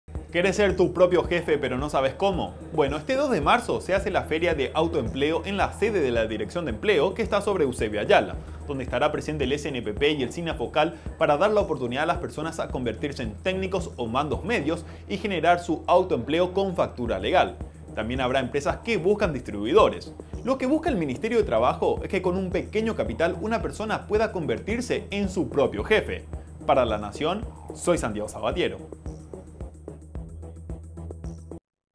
¿Querés ser tu propio jefe?
Este 2 de marzo, se hace la feria de Autoempleo en la sede de la Direccion de Empleo, que está sobre Eusebio Ayala.